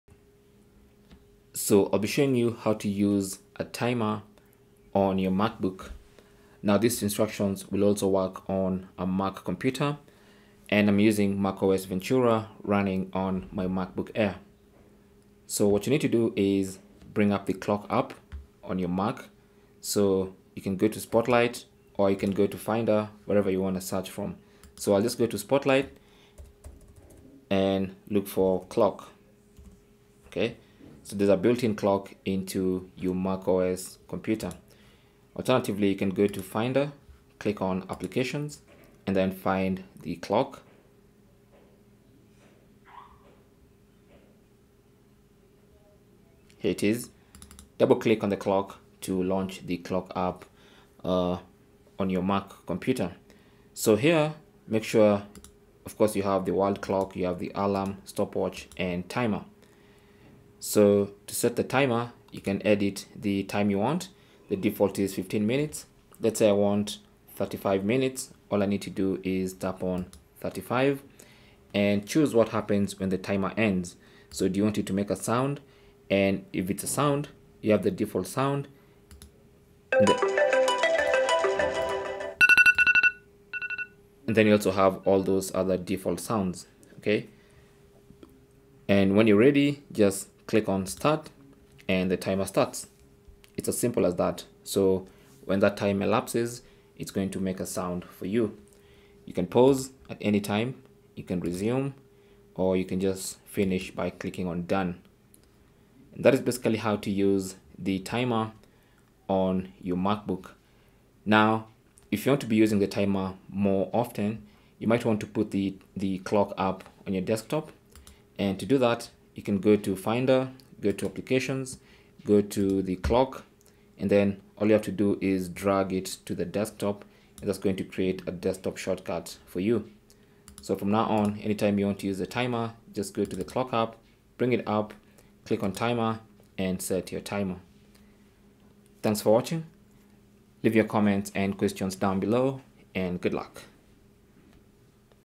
MacOS Ventura: How To Set A Timer On Mac/MacBook
Here's how to set a timer on your Mac or MacBook.

Setting a timer on your Mac has never been easier, and with the new macOS Ventura, you'll have even more flexibility and options to suit your needs. Whether you need a reminder for a task, want to limit your screen time, or simply want to time your cooking in the kitchen, this tutorial video will guide you on how to set a timer on your Mac or MacBook.

For one to use a timer on Mac, you need to access "Clock" on your Mac or MacBook. You can either use the Menu bar or Spotlight Search feature on your device. On Spotlight, tap on the search bar and type "Clock".

Click on the clock from the search results that appear. This will now launch the clock application on your Mac/MacBook. On the application, you'll see different tabs i.e. World Clock, StopWatch, Alarm, and Timer.

Click on "Timer" and you'll see a countdown timer interface. Click on the timer interface and select the amount of time you'd like to set. Then, click on "When the timer ends" to customize what will happen after the countdown is done.

Once you've set the desired time and made any necessary customizations, click on the "Start" button located at the bottom-right corner of the Timer tab. The timer will begin counting down, and you'll see the remaining time displayed in the timer interface.

During the countdown, you can pause the timer by clicking on the "Pause" button located at the bottom-right corner of the Timer tab. To stop the timer completely, click on the "Done" button (X icon) next to the pause button. This will reset the timer back to its original duration.

With this, you can use the timer on your Mac or MacBook effectively.

Galaxy S23 Ultra DUAL SIM 256GB 8GB Phantom Black:

Samsung Galaxy A14 (Factory Unlocked):

Cell Phone Tripod Adapter